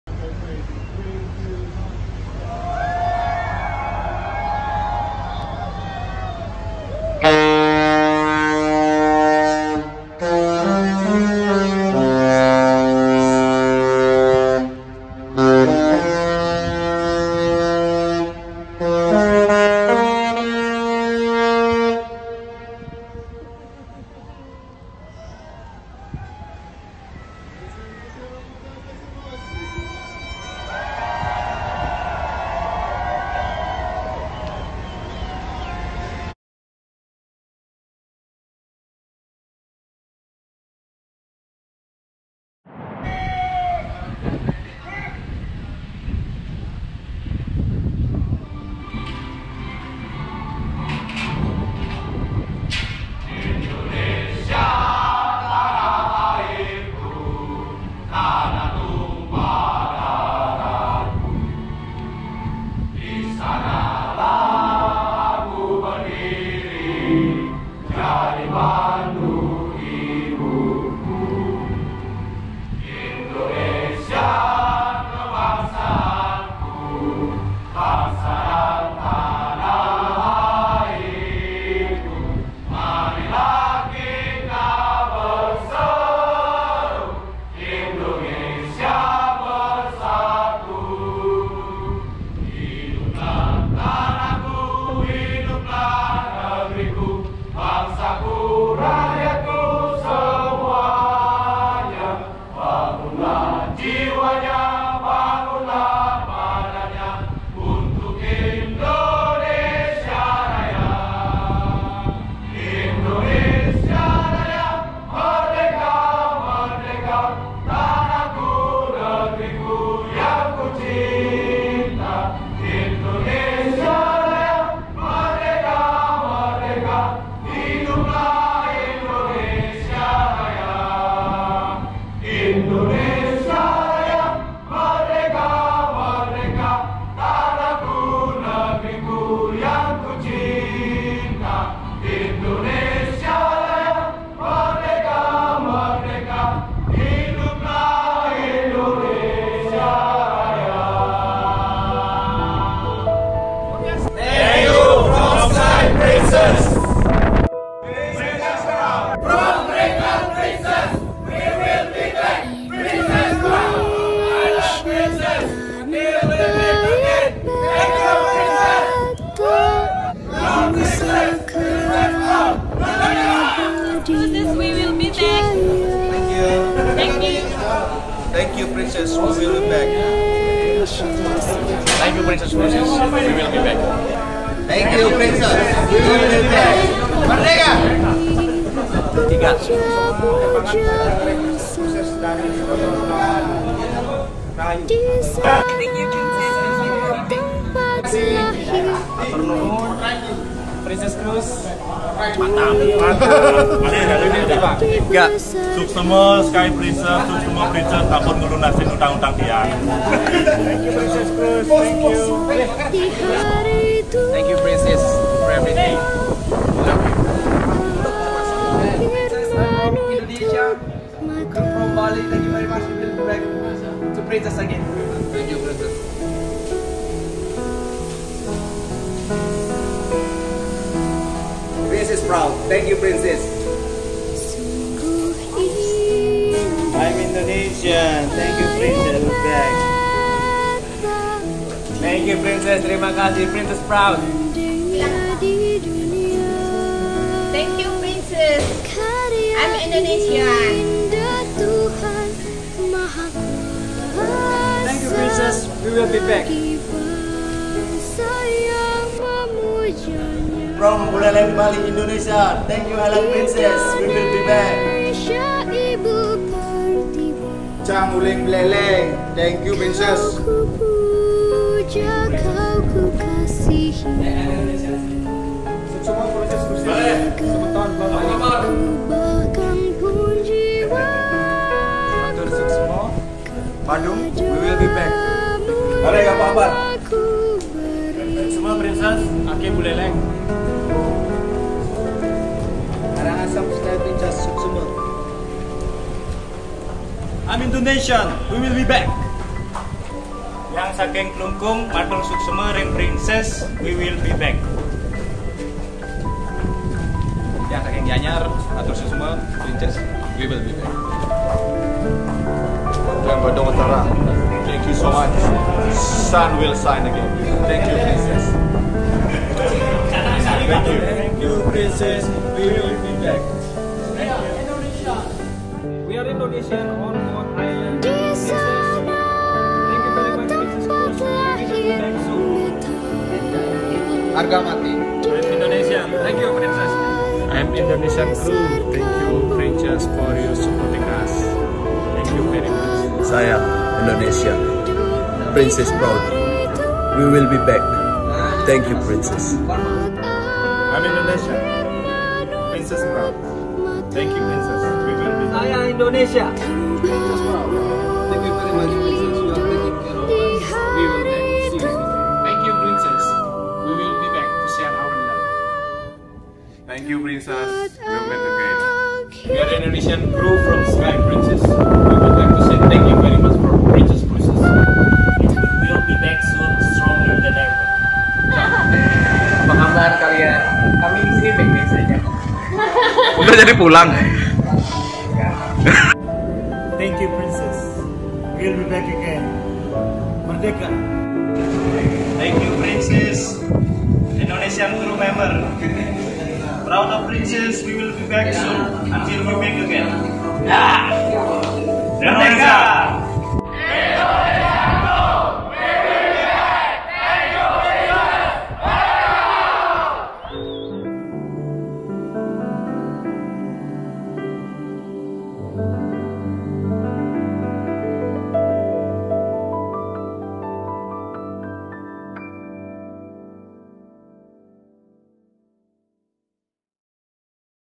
PRINCESS INDONESIAN CREW
Repatriation Princess Indonesian crew members family of Sky Princess, Regal Princess, Island Princess, Caribbean Princess and Crown Princess June 10th 2020 by Island Princess